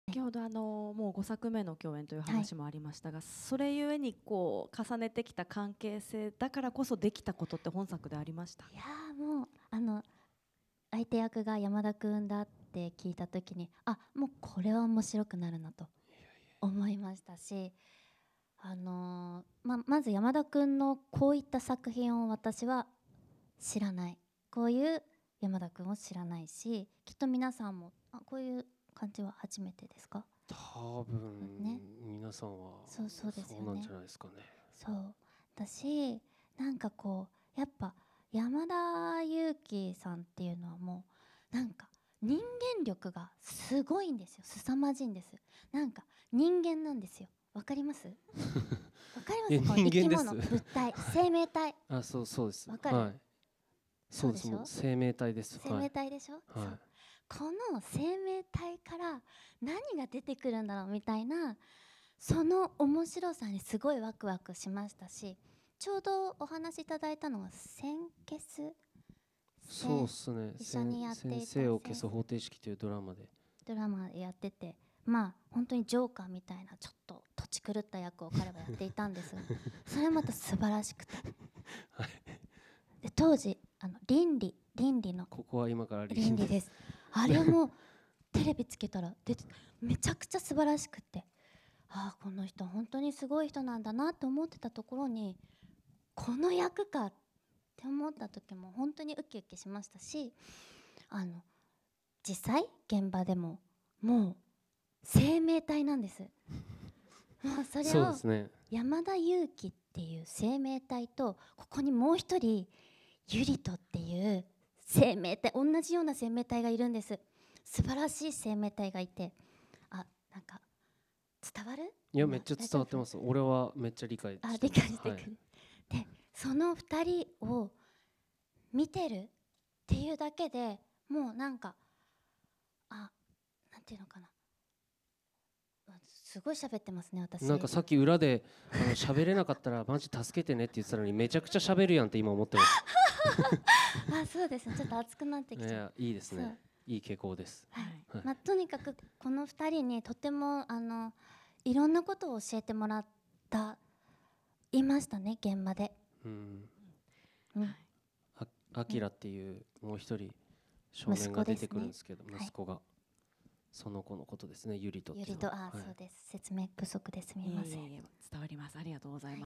松本まりか、“天然”表現連発で山田裕貴を絶賛！ 映画「夜、鳥たちが啼く」完成披露舞台あいさつ
俳優の山田裕貴さん、女優の松本まりかさんが11月17日、東京都内で行われた映画「夜、鳥たちが啼く」（城定秀夫監督、12月9日公開）の完成披露舞台あいさつに登場した。